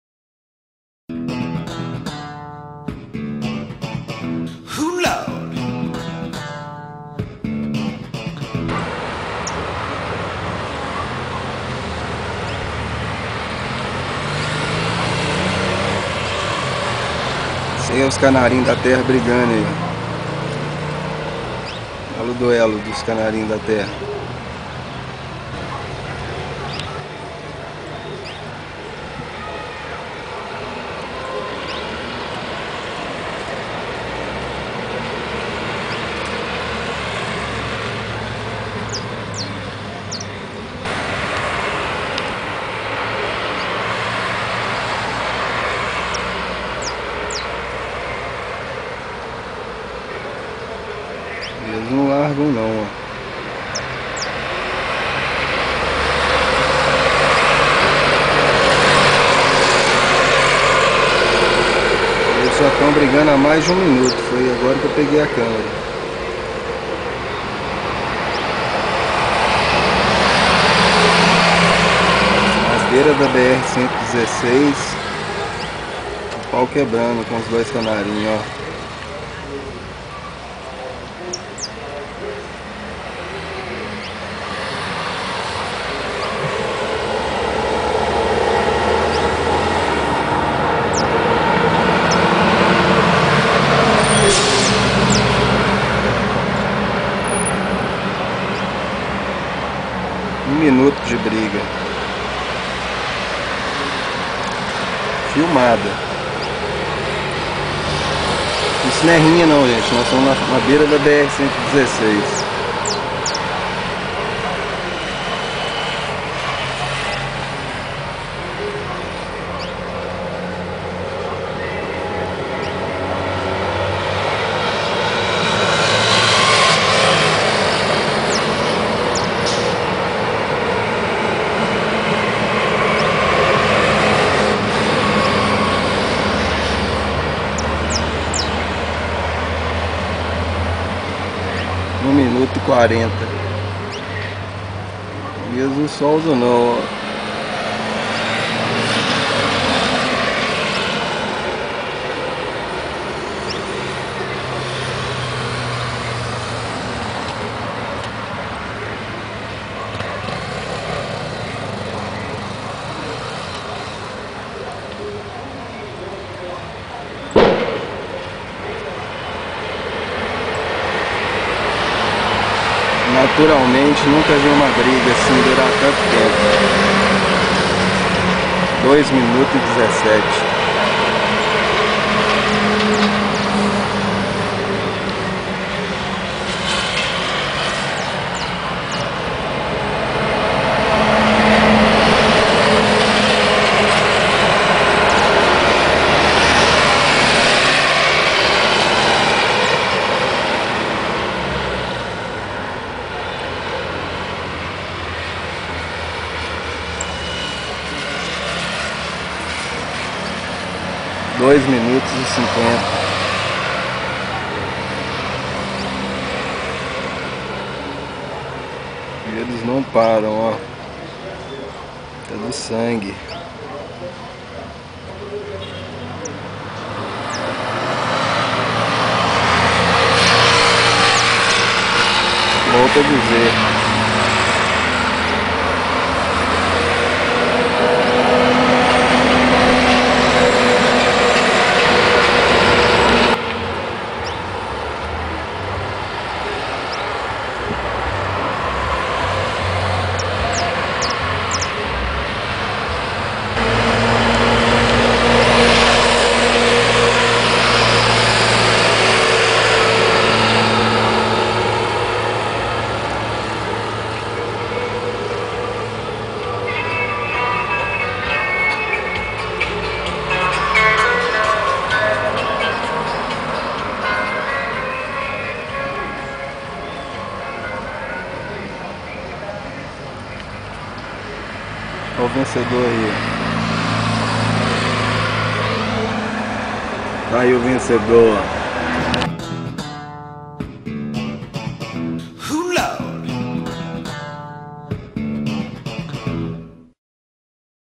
Incrível briga de Canários-da-terra-verdadeiros pela fêmea
Briga de canários da terra verdadeiro verdadeiro MMA das aves